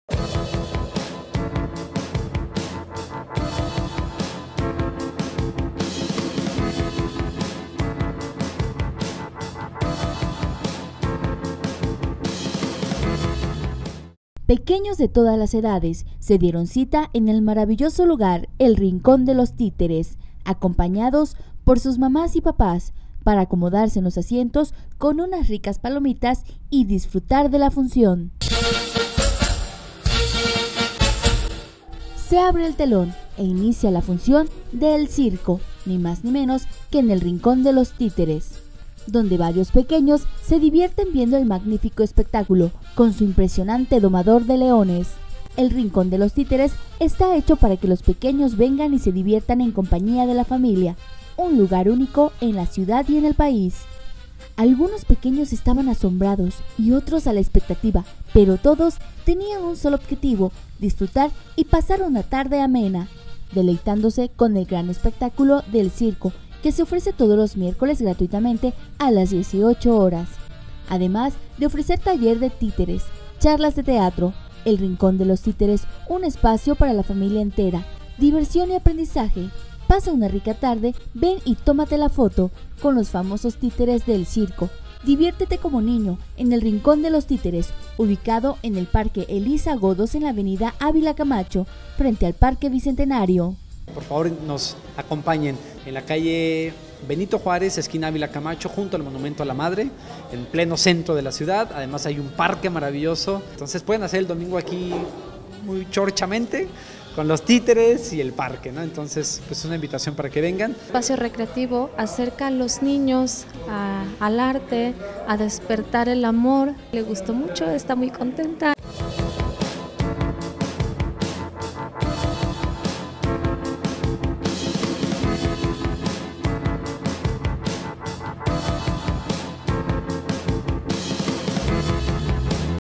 El Rincón de los Títeres en Xalapa, un espacio de convivencia familiar para ver "El Gran Circo"
"El Gran Circo" es una de las obras que se presentan todos los miércoles gratuitamente en el teatro El Rincón de los Títeres a las 18:00 hrs.
Hechos para disfrutar con la familia, donde además, se ofrecen talleres de títeres y charlas sobre teatro, sin lugar a duda una excelente opción para convivir con la familia.
Este teatro está ubicado en el Parque Elisa Godos, en la Avenida Manuel Ávila Camacho, frente al parque Bicentenario de la ciudad de Xalapa.